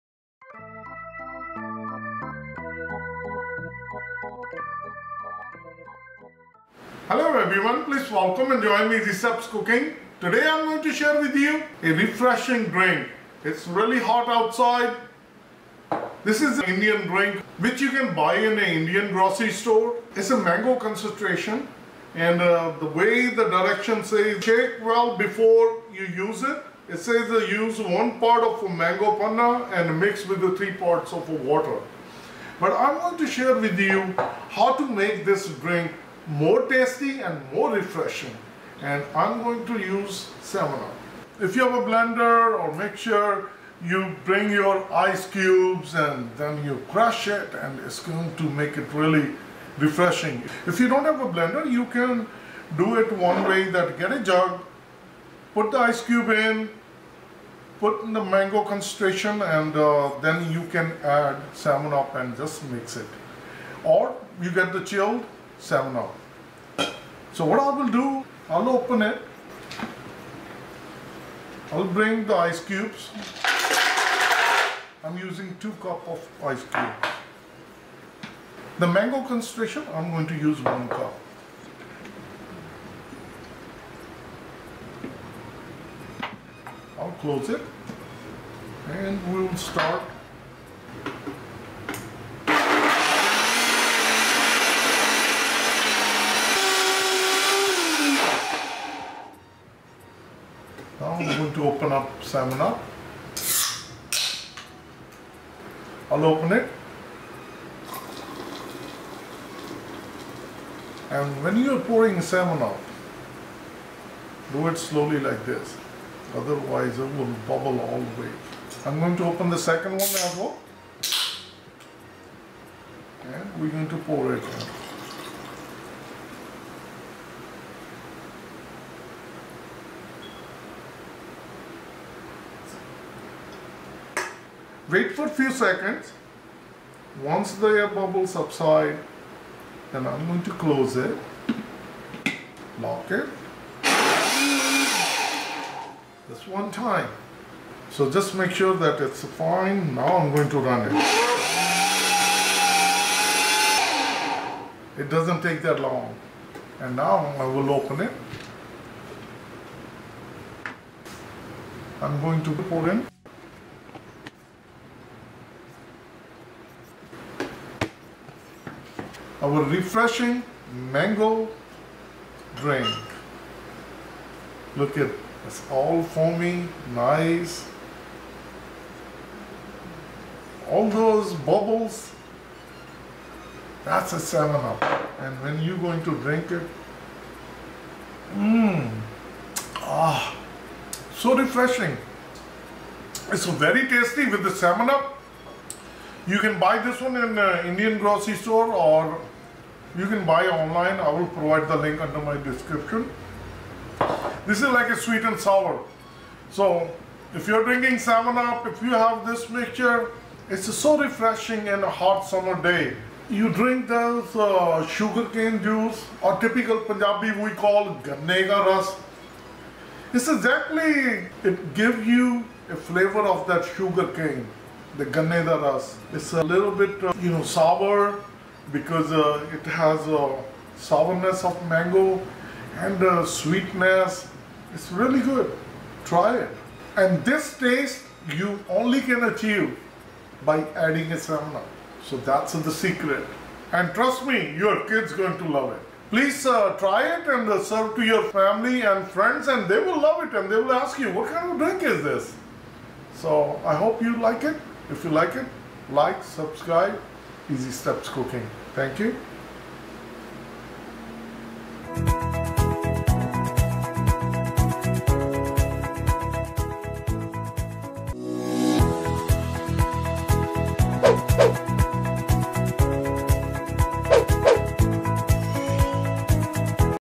Mango Panna Drink | Refreshing Indian Drink | Green Mango Drink Recipe
Mango Panna Drink | Refreshing Indian Drink | Sugarcane Drink | Green Mango Drink Recipe
Ingredients List:
1= cup Mango Panna
2= cup Ice Cubes
2= 7up

Montel Williams HealthMaster Elite Fruit & Vegetable Blender Emulsifier | 1200W
Stainless Steel Blender